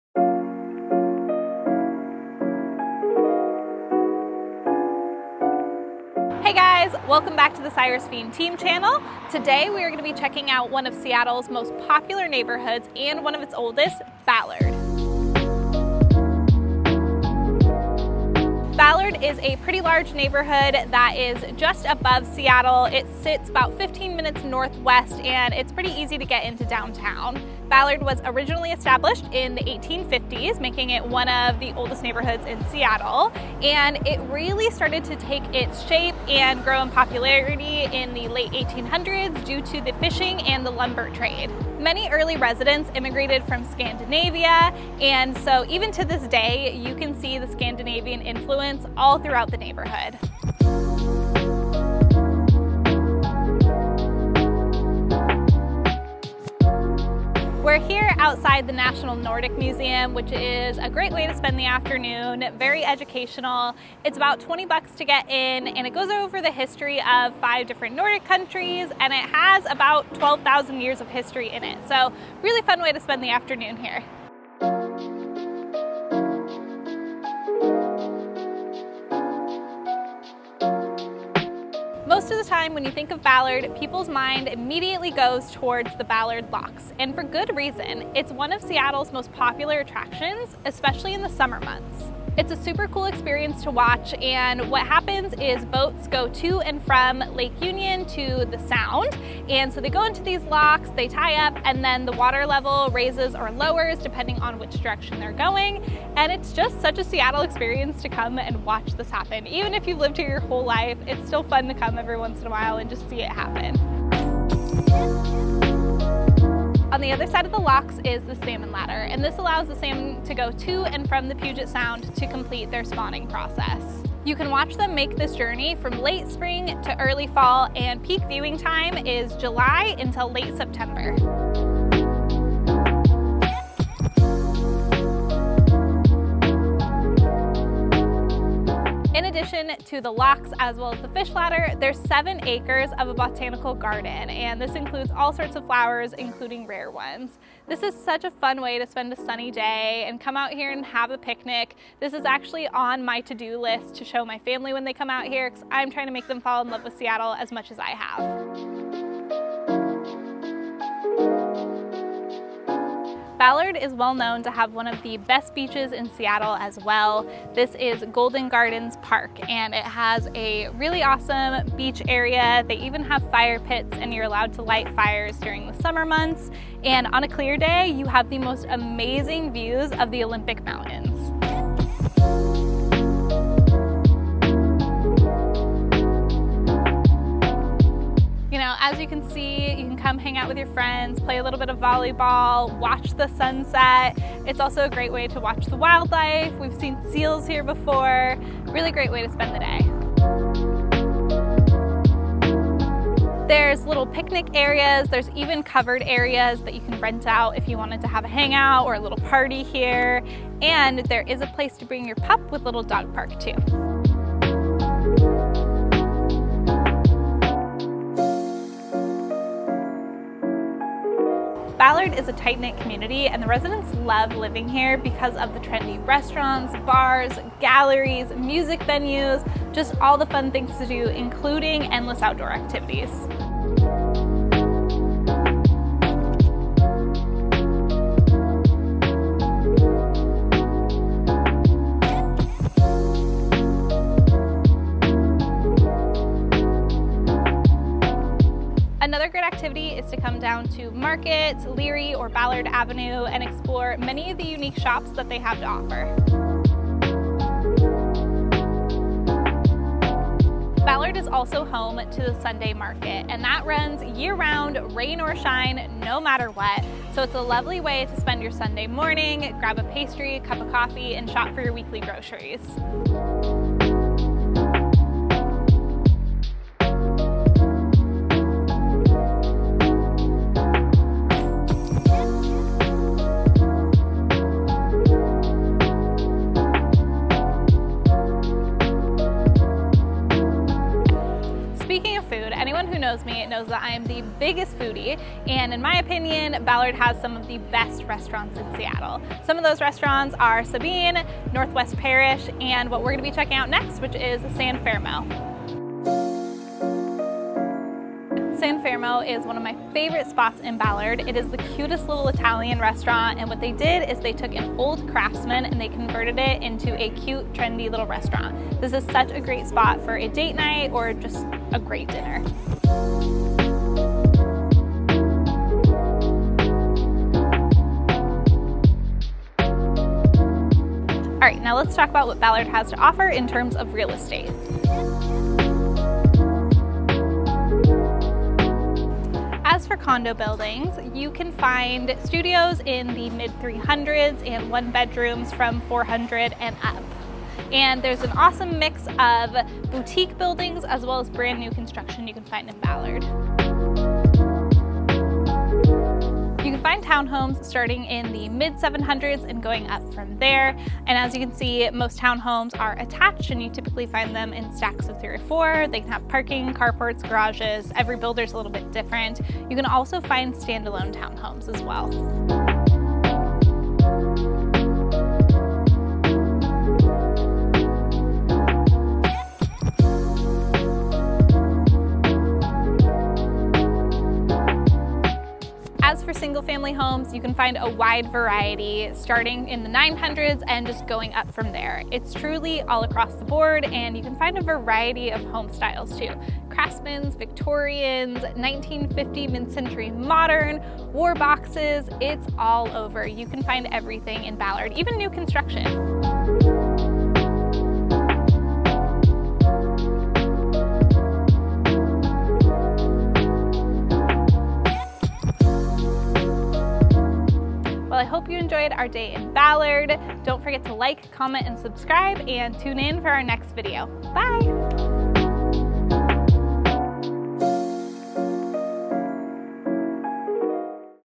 Seattle Neighborhood Tour: Ballard
Join Tarice as she walks you through what it is like to live in Ballard. She covered everything from housing costs to watching the Salmon ladder at the Ballard Locks and even exploring the Sunday Market. Tarice has you covered!